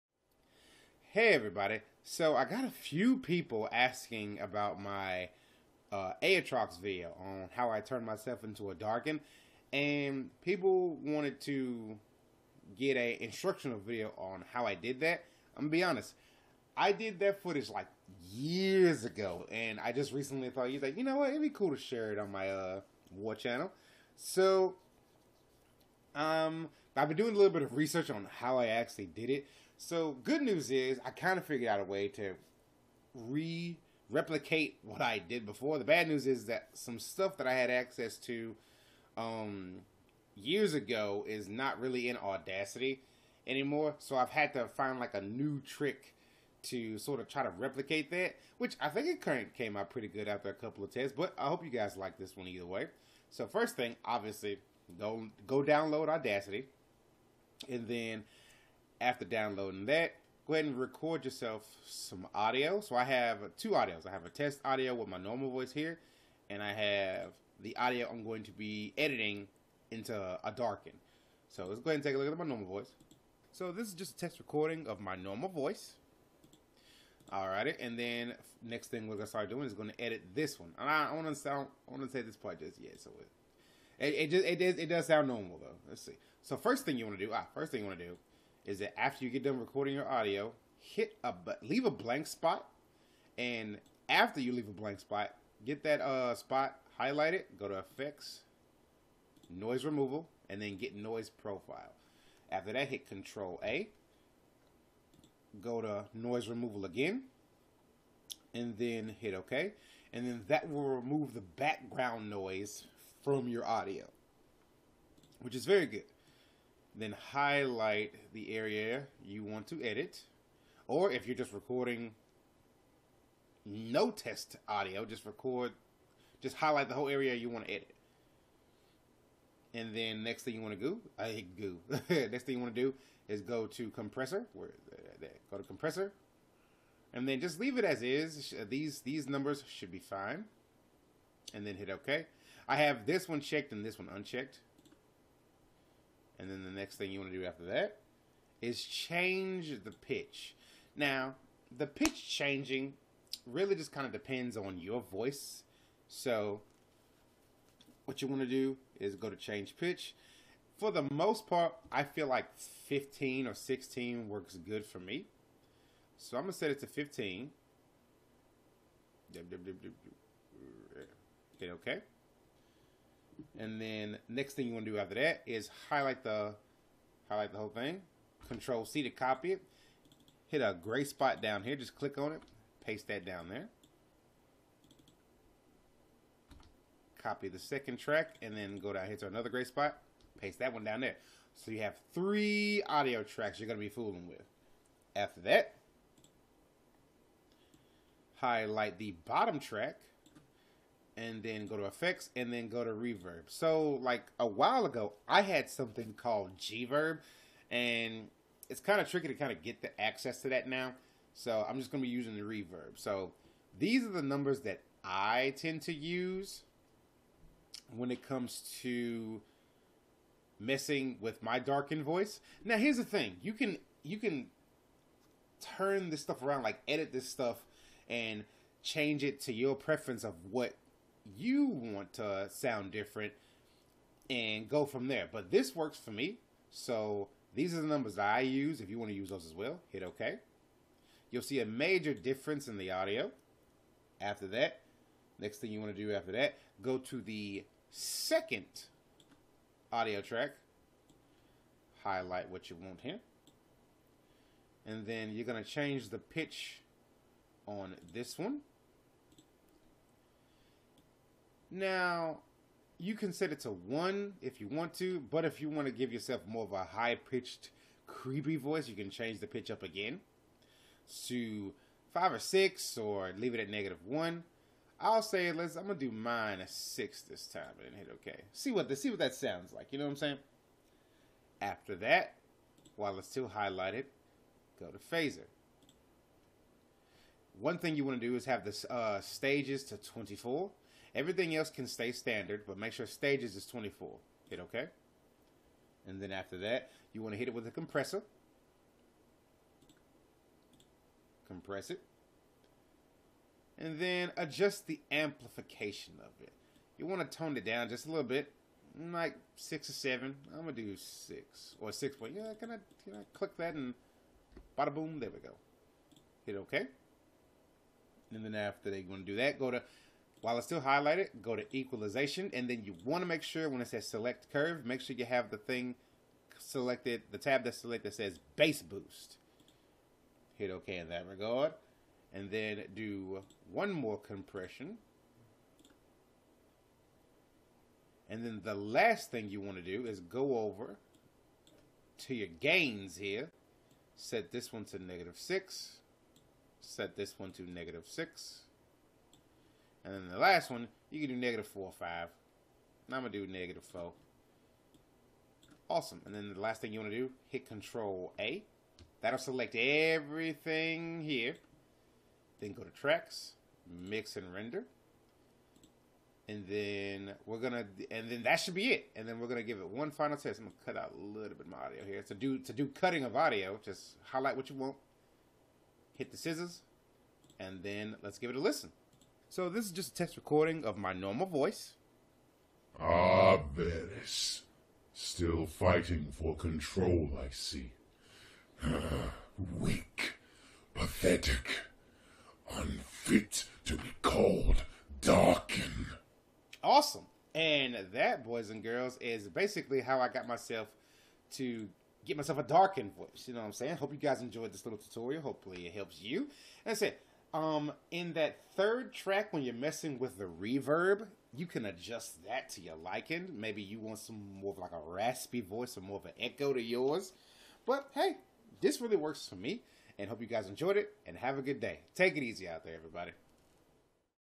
How To Make Your Voice Sound Like A Darkin Using Audacity ( Aatrox, Rhaast, Varus Race)
Well people asked for it and I wanted to oblige.  Like I say in the video I had an older version of Audacity so I was missing certain plug-ins I use to have so I tried my best to replicate what I did and I hope you all enjoy the tutorial. :)